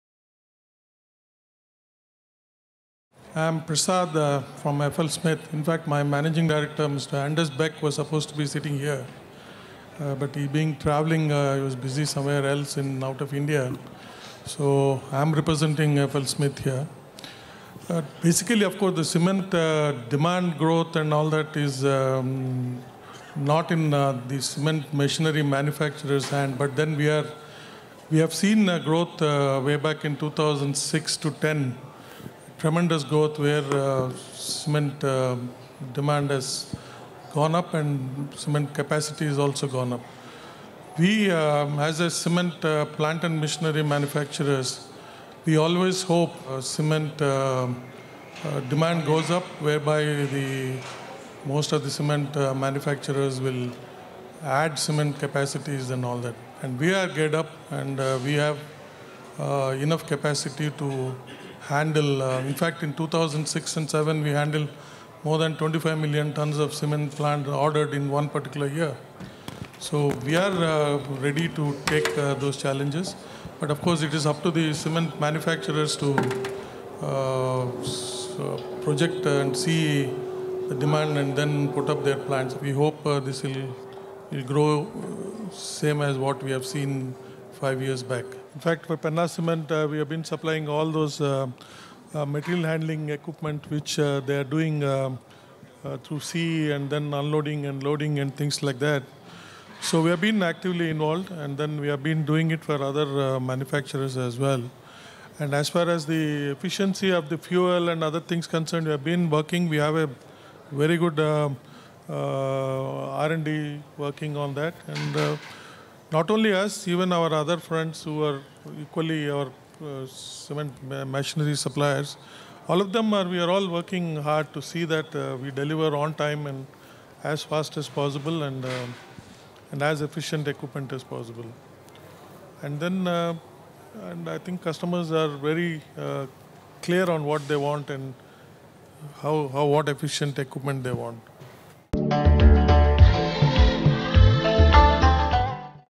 DV Prasad Advisor FLSmidth & Co | 5th Indian Cement Review Conference Cement Expo 2018
DV Prasad, Advisor, FL Smidth & Co talks at the 5th Indian Cement Review Conference 2018. He is hopeful of Cement Industry being the way it was 5 years ago. FL Smidth has been working with Penna Cements in providing various equipments